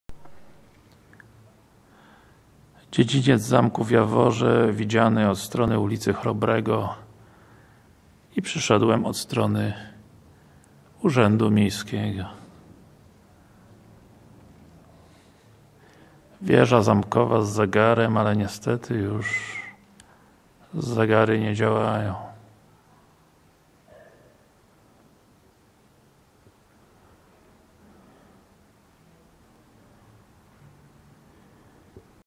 Jawor Zamek widziany od strony ulicy Chrobrego
Jawor Zamek widziany od strony ulicy Chrobrego. Widać nieco zniszczoną zamkową wieżę z zepsutymi zegarami. Więcej o tych okolicach na stronie Jawor jest niedaleko Wrocławia więc stanowi ciekawe miejsce na weekendową wycieczkę.